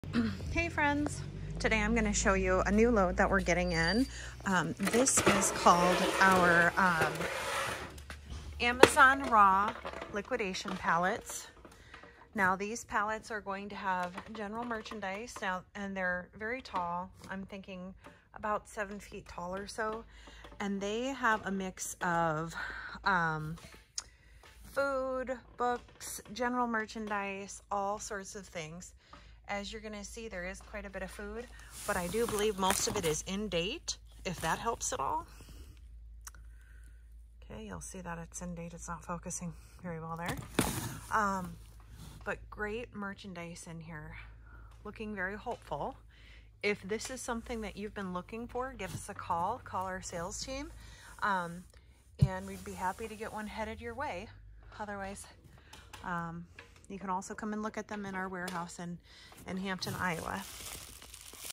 AMZ Raw High piece count truckload
7' gaylords filled with general merchandise, books, food and more!  Straight from the AMZ facility.  No sorting!  Find some gems in these loads.